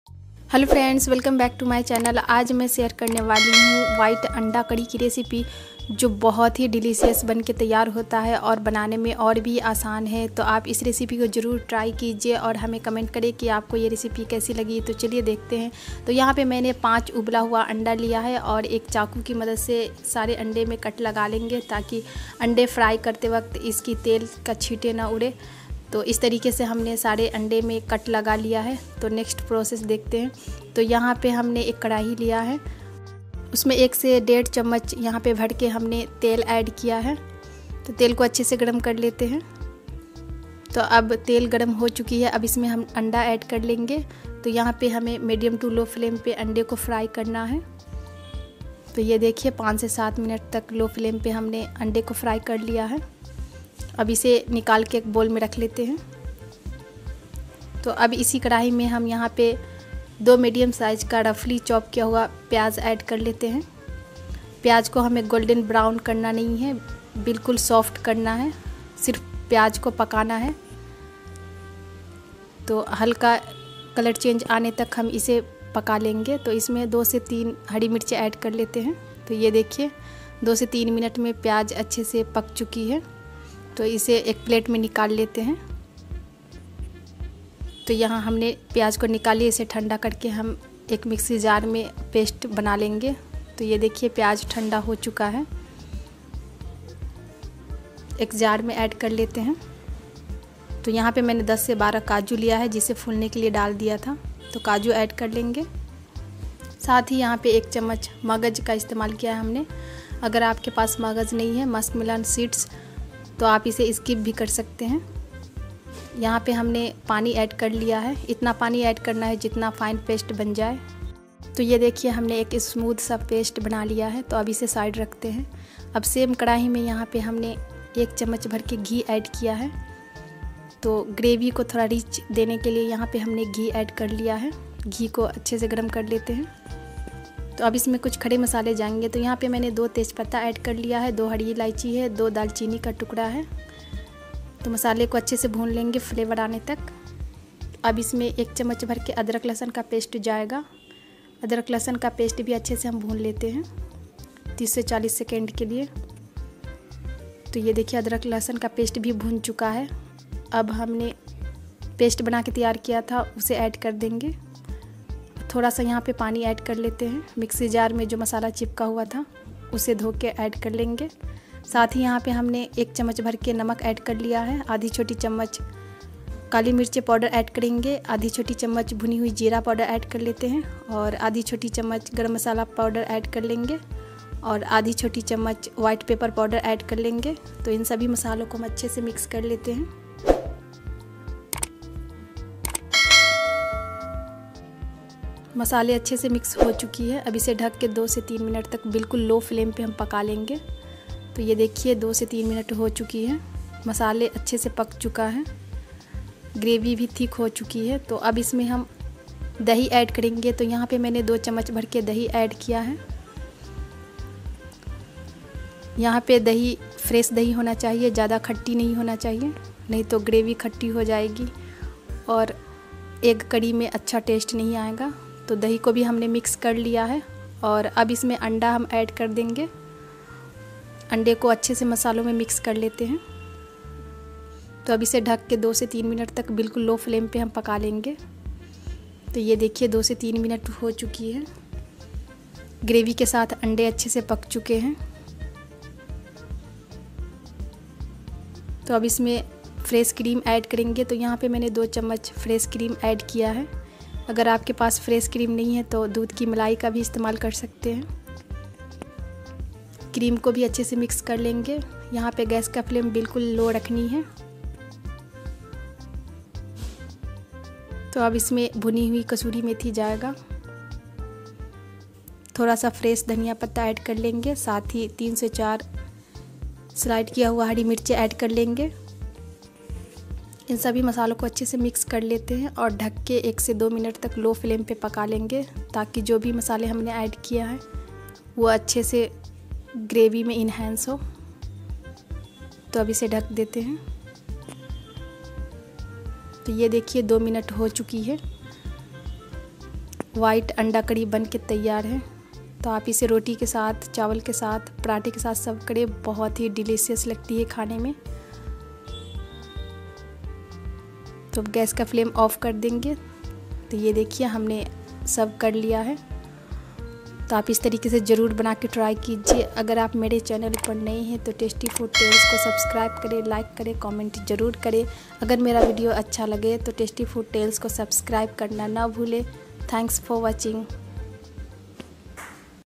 Shahi Egg Curry❤💥| शाही अंडा मलाई करी | Quick & easy White Egg Curry | Tasty Food Tales
Hello दोस्तों आज मैं आप लोगो के साथ शेयर करने वाली हु वाइट अंडा करी जो बनाने में बहुत आसान है और खाने में बहुत ही स्वादिस्ट तो आप इस रेसिपी एक बार जरूर बनाये और अपने फॅमिली और फ्रेंड्स को खिलाये और साथ ही शेयर जरूर करे अगर मेरा वीडियो अच्छा लगे तो चैनल को सब्सक्राइब जरूर करे

Press bell icone🔔🔔🔔🔔🔔

Serve for 3-4 peoples

Ingredients :-सामग्री
5 Boiled egg-- उबला हुआ अंडा
2 mid size ruffle chopp onion--- दो मध्यम साइज कटा हुआ प्याज़
3 green chilli ---हरी मिर्च
Oil for egg fry 2 tsp---  तेल अंडा फ्राई करने के लिए
1 tsp ghee --घी
Whole garam masala-- खरे गरम मसाला
Ingredients for white paste:- वाइट ग्रेवी के लिए सामग्री
12 Soaked cashews-- भिगोया हुआ काजू
1 tsp muskmelon seeds--- खरबूजे की बीज (मगज )
Cooked onion And green chilli--- पका हुआ प्याज़ और हरी मिर्च में पानी दाल कर पतली पेस्ट बना लेंगे
Add water and make it smooth paste

Salt as to test--- नमक
1/2 tsp cumin powder ---जीरा पाउडर
1/2 tsp black pepper powder ---काली मिर्च पाउडर
1/2 tsp white pepper powder--- सफ़ेद मिर्च पाउडर
1/2 tsp garam masala powder-- गरम मसाला पाउडर

Add egg and cook 3-4 min on low flam ग्रेवी को अच्छे से पकाने के बाद अंडा ऐड कर देंगे
2 tsp fresh curd--- दही
2 tsp fresh cream-- मलाई
1 tsp roasted kasuri methi--- भुनी हुई कसूरी मेथी
Fresh coriander ---हरी धनिया बारीक़ कटा हुआ
3 slite green chilli--- तीन स्लिट किया हुआ हरी मिर्च
Cook 2 min and---  दो मिनट धीमी आंच पर पकने देंगे
Ready to serve ---वाइट अंडा करी तैयार है आप इसे रोटी पराठा नान के साथ परोसे बहुत ही स्वादिष्ट लगती है